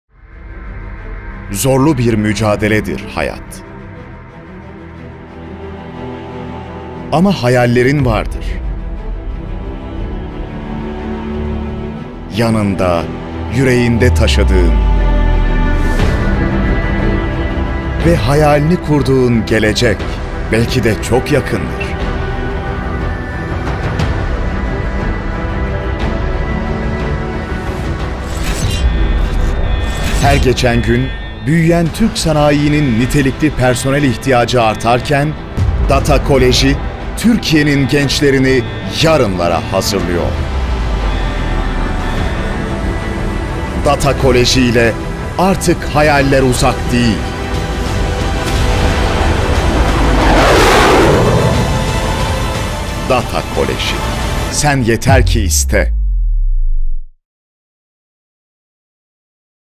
Sen Yeter ki İste! | Data Koleji Havacılık Lisesi Bursa
Her geçen gün büyüyen Türk sanayisinin nitelikli personel ihtiyacı artarken Data Koleji Türkiye'nin gençlerini yarınlara hazırlıyor.

Data Koleji Havacılık Lisesi ile artık hayaller uzak değil!

Sen Yeter ki İste!